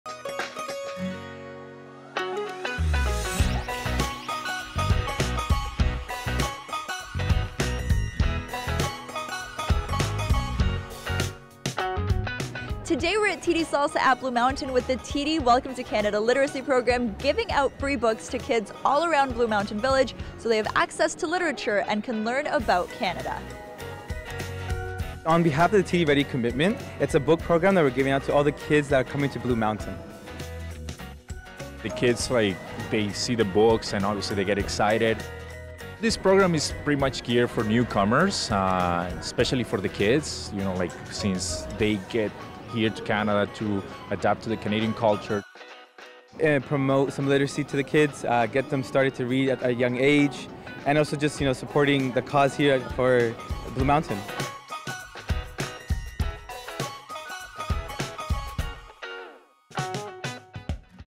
TD Welcome to Canada at the 14th TD Salsa At Blue Mountain!
Every year, the TD Salsa In Toronto festival is proud to partner with TD Bank Group & TLN Television for the TD Welcome To Canada initiative! This amazing program foster an understanding of our beautiful country and build literacy awareness among new Canadian families. 📚✨

Take a look at how it went at the 14th TD Salsa At Blue Mountain festival, and find it again on July 6 & 7 at the 20th TD Salsa On St. Clair festival! Come say hi and grab free children’s books for all ages!